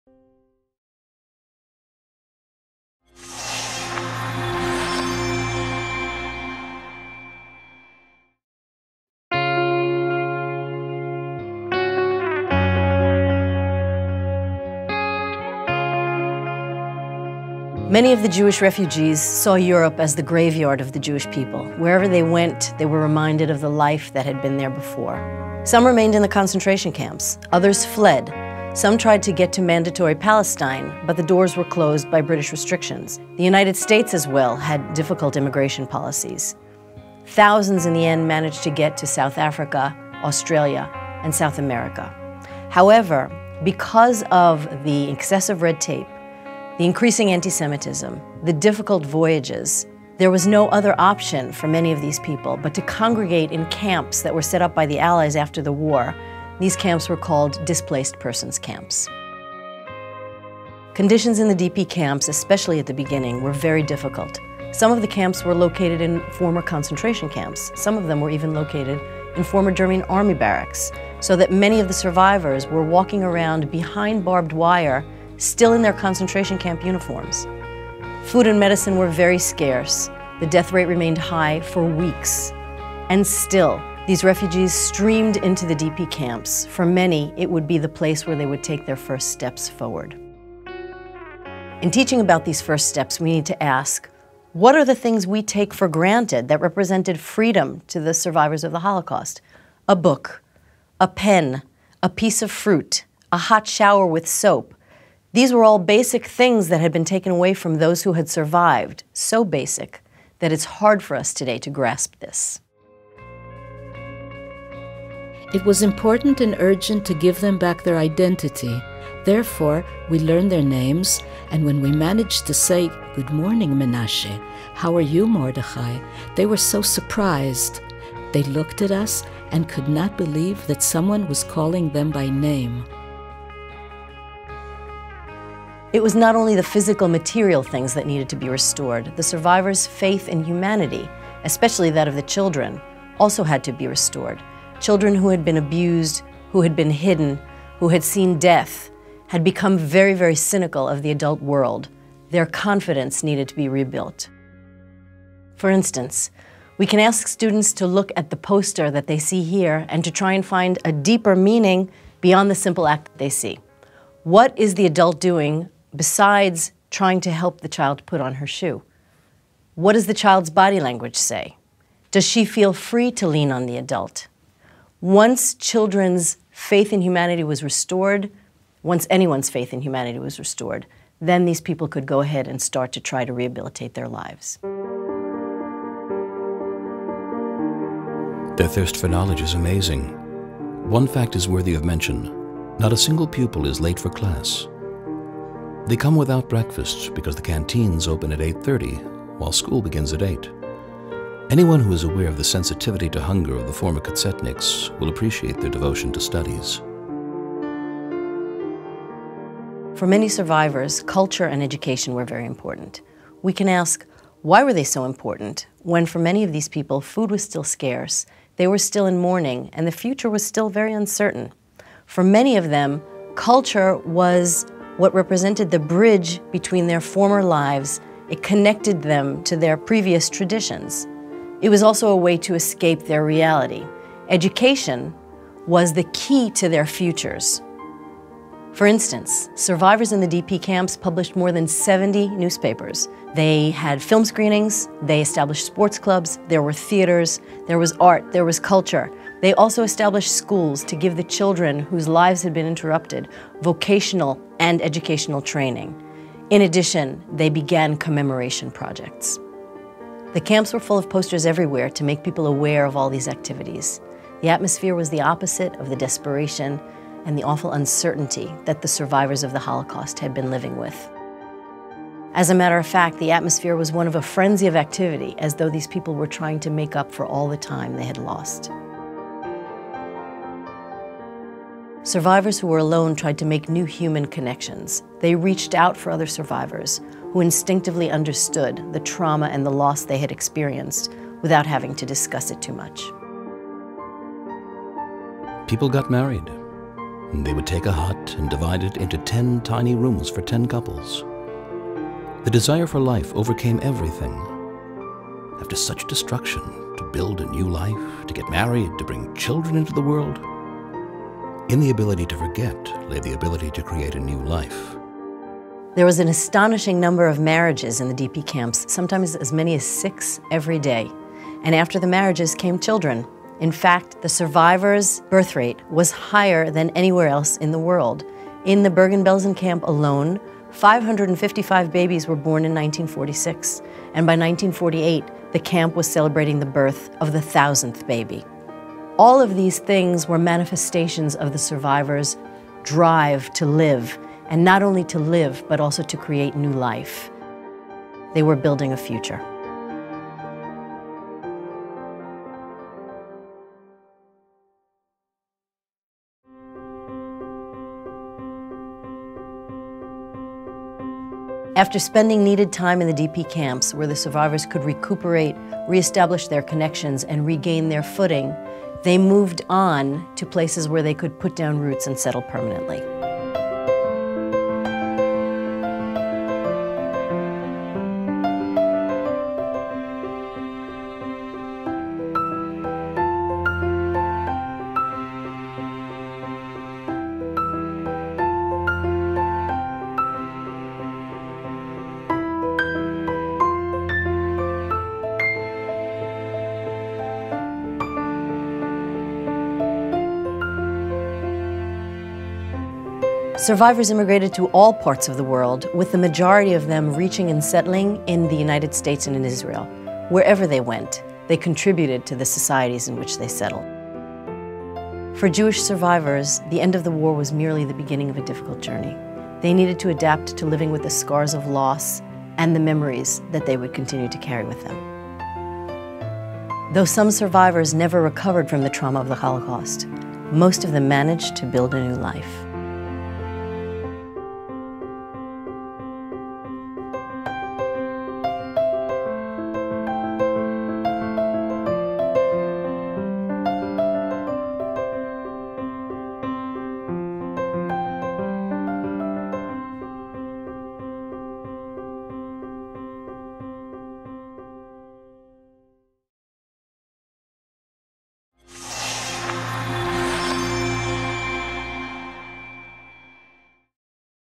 Holocaust Survivors – First Steps in the DP Camps and a New Beginning
"This video is part of the Holocaust Education Video Toolbox.

In the video, "Holocaust Survivors – First Steps in the DP Camps and a New Beginning", ISHS staff member Sheryl Ochayon presents the story of the survivors, following the fundamental dilemma - "What Now?" - through to life and culture within the DP camps. She outlines the reality and remarkable phenomena within the DP camps, as well as their human significance in restoring a sense of personal identity and early steps towards a new beginning. The materials discussed in this video are available on our website and in teaching units produced at the ISHS.

Sheryl Silver-Ochayon is a staff member at the International School for Holocaust Studies, Yad Vashem.

part 1: The Return to Life in the DP Camps 00:00
part 2: A New Beginning 5:56

Archival footage and photographs:
Yad Vashem Photo Archive.
Yad Vashem Film Archive.
Yad Vashem Museum Collection.
Steven Spielberg Film and Video Archive, United States Holocaust Memorial Museum.

The Joint Archives, New York
Kibbutz Haogen’s Archive. Filmed by Yithak Herbert

Material and quotes from:

Study kit Return to Life - The Holocaust Survivors: From Liberation to Rehabilitation, produced by Beth Hatefutsoth, Tel Aviv; Ghetto Fighters' House, Kibbutz Lohamai Hagetaot; Yad Vashem, Jerusalem;
Tel Aviv, 1995.

Yehudit Hemendinger, Perspectives on Holocaust Survivors. A Psychological Approach to the Study of the jewish People, Miriam Reiter-Tzedek, ed., The Institution for the Study of the Jewish People, May, 1984
Pinhas varshavsky. in Idit Witman, Unser Stimme (Our Voice), the first journal of the survivors, Gesher 4, 1987.

2. The Anguish of Liberation - Testimonies from 1945, edited by Y.
Kleiman and N. Springer- Aharoni, Yad Vashem, Jerusalem, 1995.

3. Yad Vashem Archives